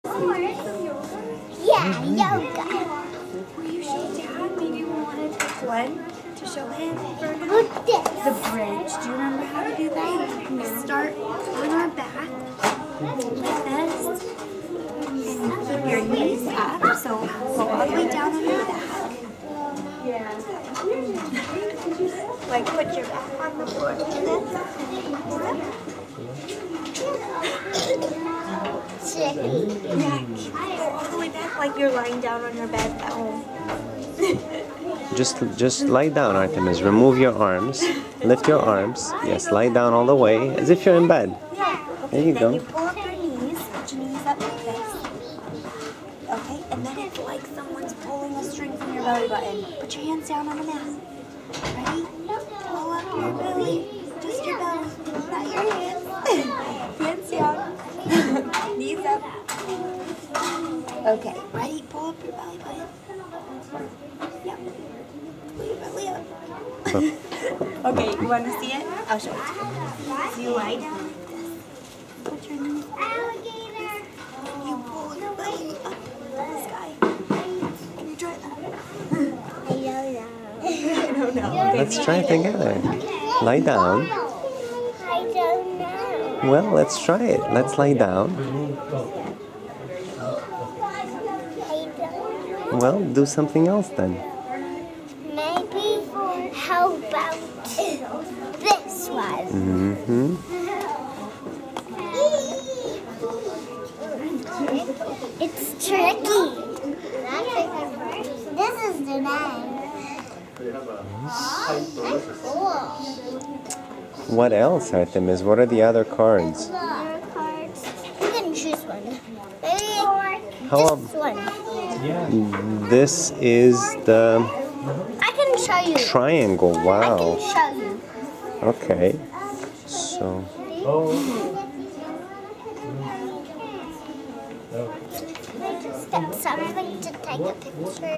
Visit to Blue Room | Cambridge Montessori School
Shadowing Artemis in the Blue Room - she shows me yoga, counting, and sorting